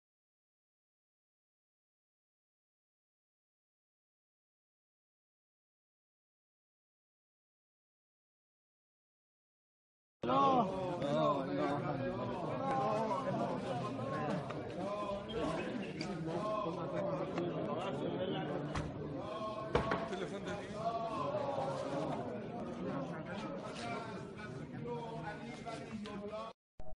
Son of Hashemi Rafsanjani prevent IRIB director to handle casket of his father
During feral of former president Hashemi Rafsanjani on Monday 9 January 2017 his son Mehdi  Hashemi Rafsanjani prevent IRIB director to handle casket of his father 1